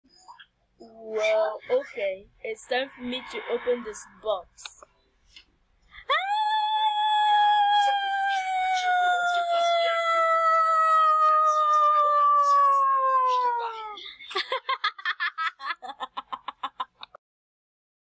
YunokiRoom Bullshit: Estella Scares Senoyu
Senoyu opens a box that she got from Cecilia, but it was revealed to be Estella.